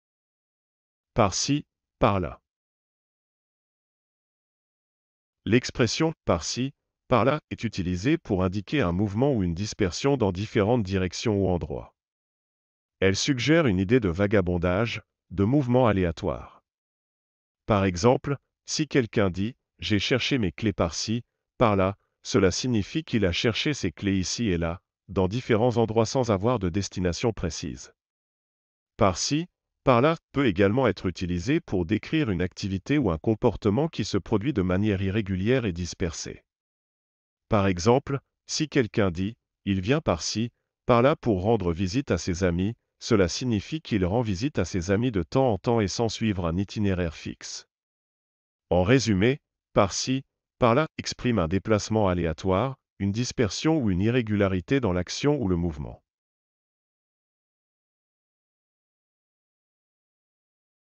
Par-ci, par-là
L'expression "par-ci, par-là" est utilisée pour indiquer un mouvement ou une dispersion dans différentes directions ou endroits. Elle suggère une idée de vagabondage, de mouvement aléatoire.
Par exemple, si quelqu'un dit : "J'ai cherché mes clés par-ci, par-là", cela signifie qu'il a cherché ses clés ici et là, dans différents endroits sans avoir de destination précise.
"Par-ci, par-là" peut également être utilisé pour décrire une activité ou un comportement qui se produit de manière irrégulière et dispersée. Par exemple, si quelqu'un dit : "Il vient par-ci, par-là pour rendre visite à ses amis", cela signifie qu'il rend visite à ses amis de temps en temps et sans suivre un itinéraire fixe.
En résumé, "par-ci, par-là" exprime un déplacement aléatoire, une dispersion ou une irrégularité dans l'action ou le mouvement.